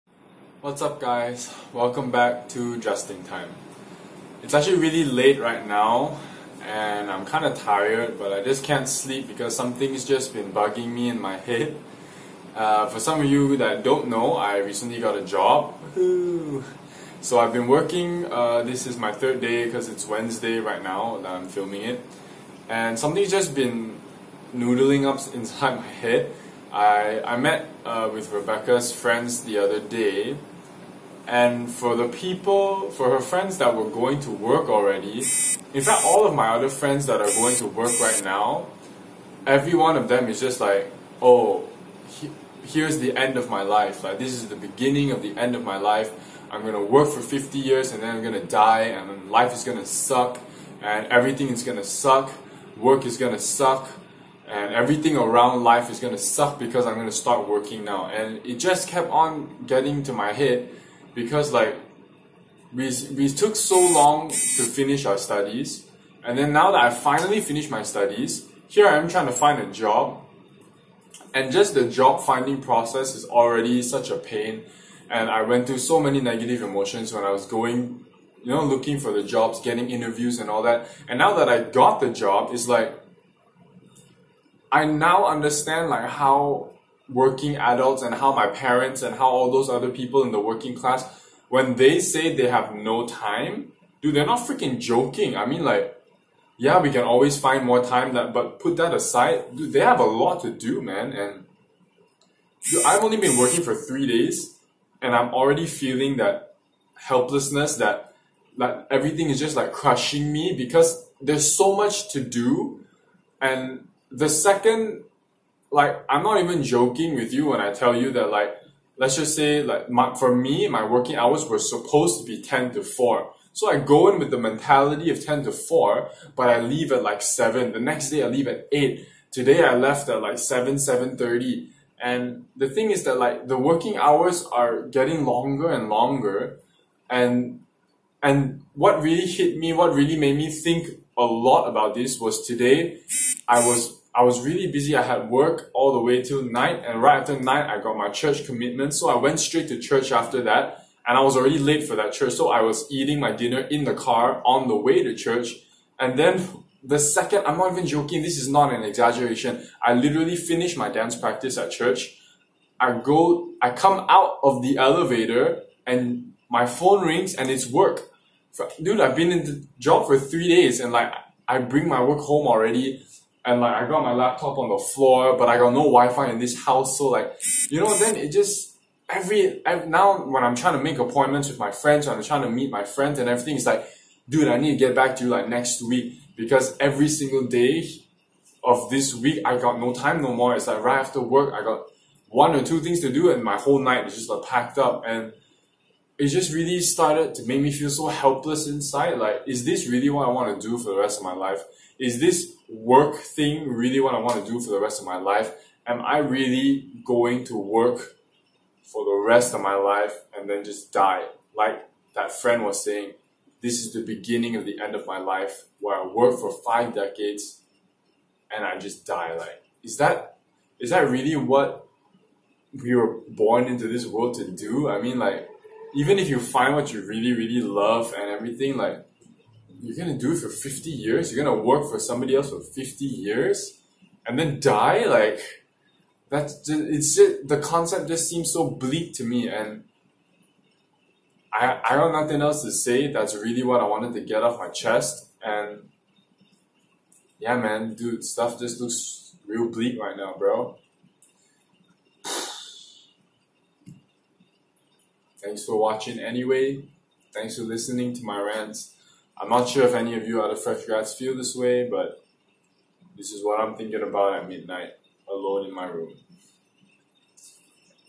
JustTalk #8: Will I work forever???
This is me being super vulnerable about how I feel (I hope my boss doesn't see this xD)
Being a fresh graduate we all have to come out to find that FIRST job. But everyone I talk to loathes it. And now that I'm doing it I wonder, will I ever earn back time for myself? Or is this just the end?